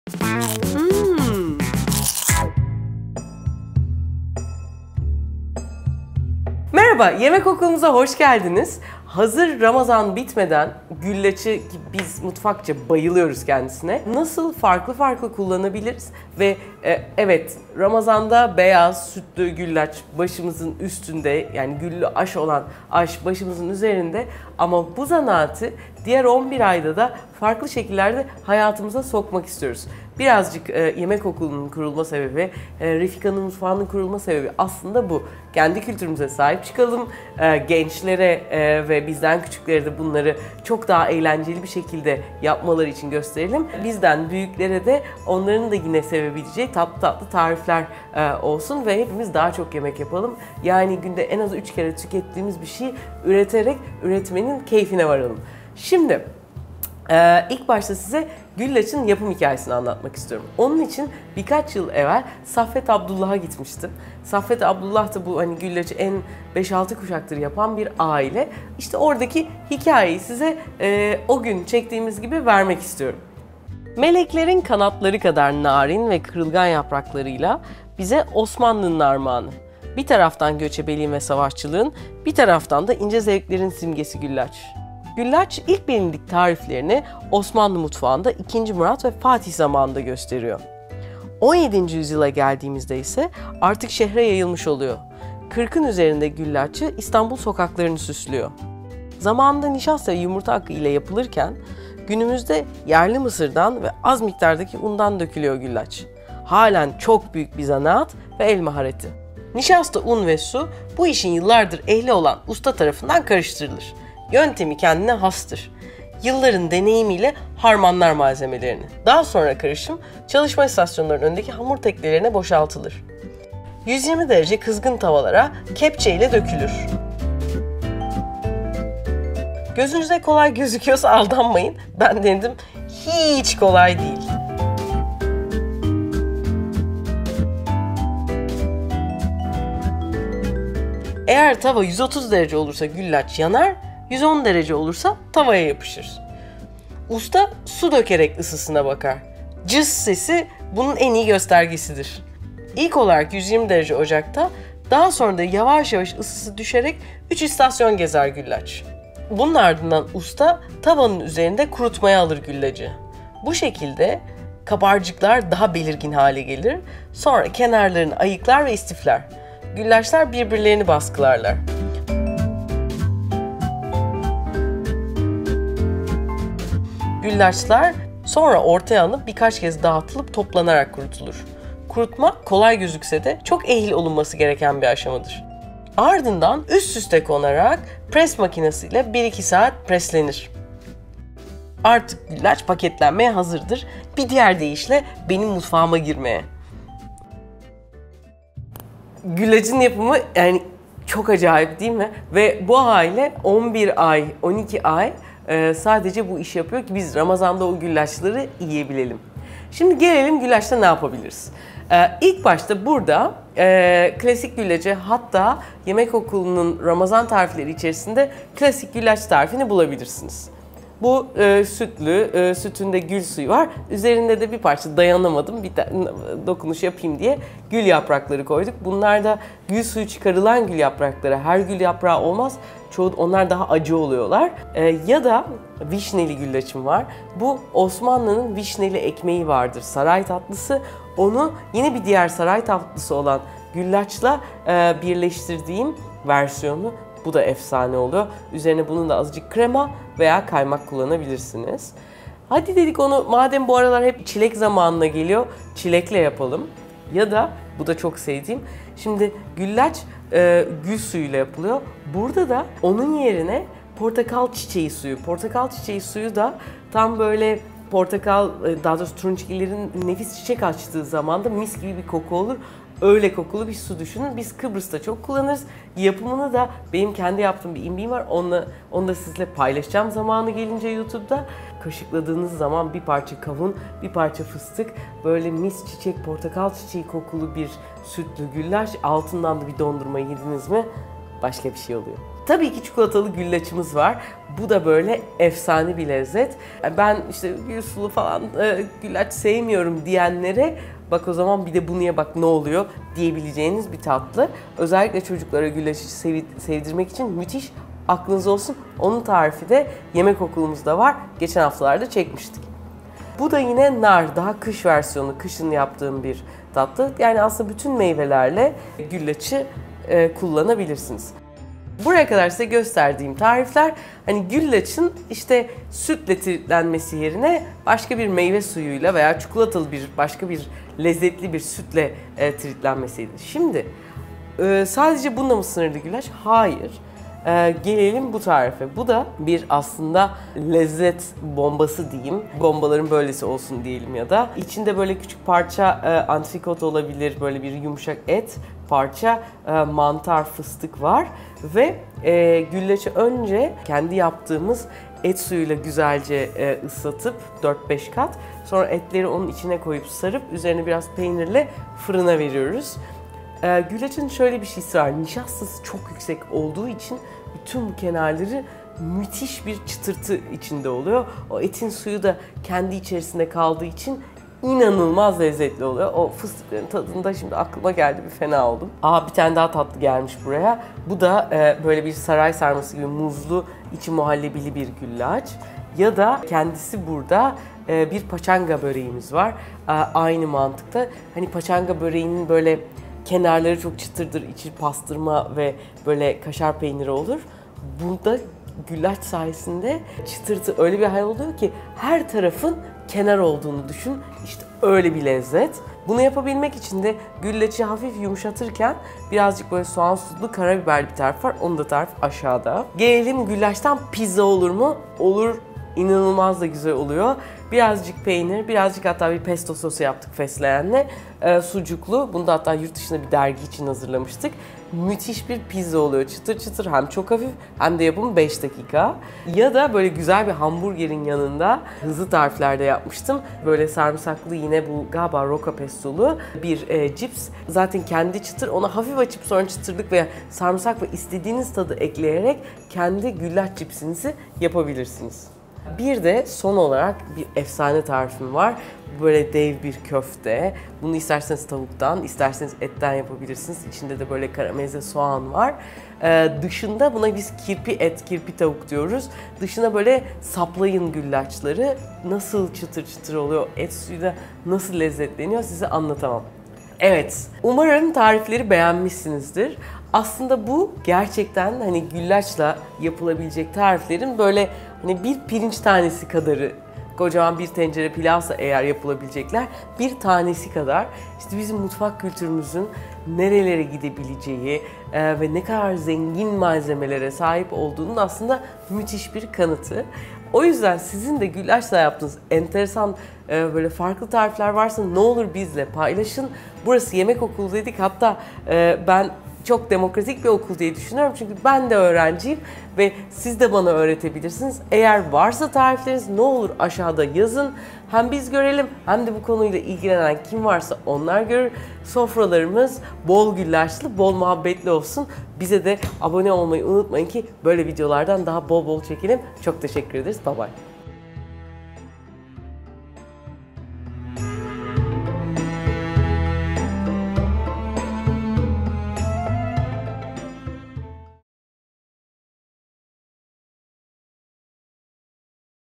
Güllacın Bilinmeyen Anatomisi | Güllaç Nasıl Yapılır ve Güllaçla Yapılabilecek Tarifler
Meleklerin kanatları kadar narin ve kırılgan yapraklar bize Osmanlı’nın armağanı. Güllaç bir taraftan göçebeliğin ve savaşçılığın bir taraftan ince zevklerin simgesi.

Zamanında nişasta ve yumurta akıyla yapılan güllaçlar, günümüzde yerli nişasta ve az miktarda un kullanılarak dökülüyor. Halen büyük bir zanaat güllaç yapmak. El maharetiyle hazırlanıyor, incelikli bir üretim aşamasından sonra bizlerle buluşuyor. Arkasındaki bu el emeği göz nuru durum bile onun kıymetini anlamamız için yeterli.

Ramazanda klasik güllaç nefis bir âdet. Ne yazık ki ramazan haricinde güllaç pek yenmiyor diye sair zamanlarda bakkallarda bulmak pek mümkün değil. Kadıköy, Üsküdar, Mısır Çarşısı gibi lokal çarşılarda bulunuyor sadece. Hazır her yerde bulunabilirken başka tarifler denemek için de kenara birkaç paket stoklayabilirsiniz.

Sizi 1881’den beri güllaç döken bir aile ile tanıştırmak istiyorum. Yüzde 85’i ramazanda yenen güllaç için aile bütün yıl harıl harıl çalışıyor. Güllaç tavalarını eski zamanlardaki kalkanlar gibi düşünebilirsiniz, ek olarak bir sapı var. Epeyce ağır olan bu kalkanları kızgın ateşin önünde bütün gün sallayan kadın ve erkeklerin elleri öpülesi.

Yapım Aşamaları;

Nişasta, un ve su bu işin yıllardır ehli olan ustalar tarafından kendine has bir yöntemle karıştırılıyor. Yılların deneyimiyle harmanlanıyor.
Elde edilen karışım tezgâhlardaki hamur tepsisine boşaltılıyor ve ardından 120 derecelik kızgın tavalara bu karışım dökülüyor. Bu o kadar ince bir ayar gerektiriyor ki, ısı 130 derece olursa güllaç hemen yanıyor, 110 derece olursa tavaya yapışıyor.
Usta tavaya su dökerek ısıyı kontrol ediyor. Her farklı dereceye göre çıkan ‘cıss’ sesi farklı oluyor. Bu da yılların ehil ustası ısının derecesine dair bir gösterge.
Güllaç, pişme macerasına 120 derecelik tavalarda başlıyor, sonra yavaş yavaş ısısı düşerek, farklı ısılardaki tavalarda, üç istasyon değiştirerek devam ediyor.
Usta, pişen güllaçları kuruması için tezgâhın üstüne alıyor. Bu kuruma işleminde, güllacın kabarcıkları daha belirgin hale geliyor. Kurutma yapılırken güllaçlar birkaç kez dağıtılıp, toparlanıyor. Kuruma işleminde güllacın neminin yüzde 12’yi geçmemesi lazım. Yani burada da ince bir hassasiyet noktası ustada olması gerekiyor.
Kurutma sırasında güllaçların fazla kenar parçaları ayıklanıyor, aynı şekle getiriliyor ve istifleniyor.
Birbiri üzerine istiflenen güllaçlar kuruma safhasından geçtikten sonra pres makinesinde bir-iki saat bekletiliyor. Böylece güllaç paketlenmeye hazır hale geliyor.

Güzel bir güllaçın 4 püf noktası;

1-Yaprakları tavaya dizerken parlak tarafı yukarı bakmalı.
2-Gülllacı ıslatacağınız şerbetin, sosun ısısı güllacın doğru reaksiyon vermesi için önemli. Güllaç fazla ısıda erir, düşük ısıda kendini istenen şekilde bırakmaz. Yoğurt mayalama ısısı olan 38-41 derece bunun için ideal.
3-Her yaprağı tek tek ıslatmalısınız.
4-Güllacın üzeri şerbetle iyice kaplanmalı, şerbet biraz da içinde yüzebilecek miktarda olmalı.

Saffet Abdullah ailesinin kısa hikâyesi;

1878’deki Osmanlı- Rus savaşı sırasında Kırım’dan İstanbul’a göç eden Abdullah Efendi, 1881’de İstanbul’da güllaç üretimine başlar. 40 yıl çalışdıktan sonra işi oğlu Saffet Efendi’ye devreder. Üçüncü kuşak üretim Saffet Efendi’nin oğulları Yalçın Arseven ve İlhan Arseven tarafından yapılır. Gittiğimizde tanıştığımız Gürsel ve Erdal Arseven kardeşlerse ailenin dördüncü kuşak oğulları. Erdal Bey işletmeci, Gürsel Bey ise doktor. Saffet Abdullah bu işin elle yapıldığındaki lezzeti vermediğine inandığından, bu güzel geleneği devam ettirmeyi vazife edinmiş.

Mutfağımızda ve Yemek Okulu’muzda neler oluyor daha yakından takip etmek için;